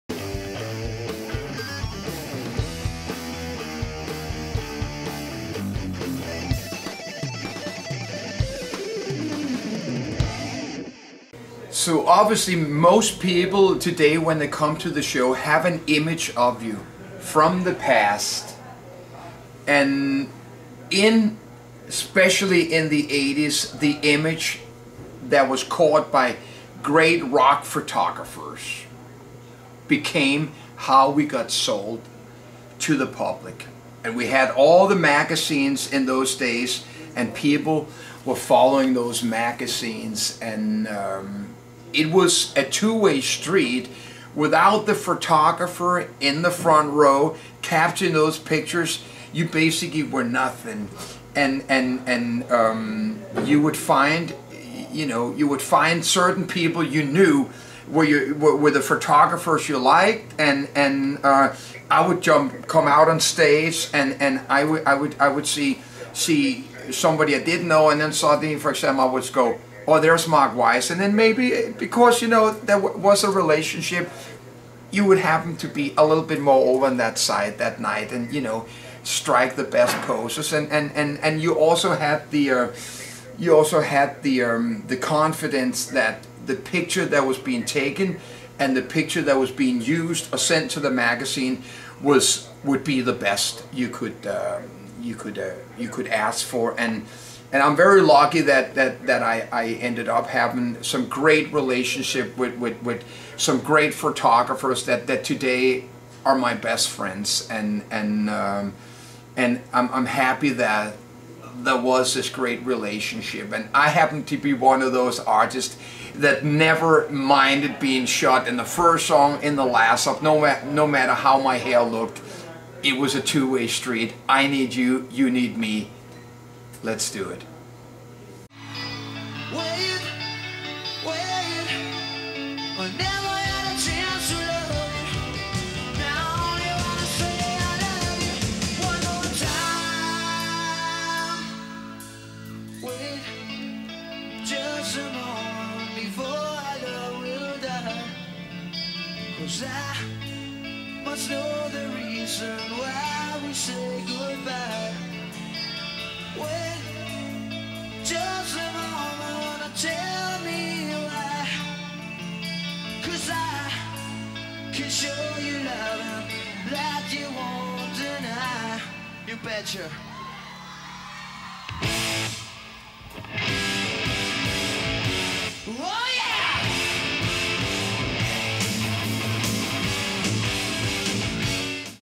MIKE TRAMP talks about THE DECADE THAT ROCKED!
MIKE TRAMP reflects on magazines and rock photography and Mark "Weissguy" Weiss iconic 80s Rock Photography book THE DECADE THAT ROCKED.  Pre-order on AMAZON now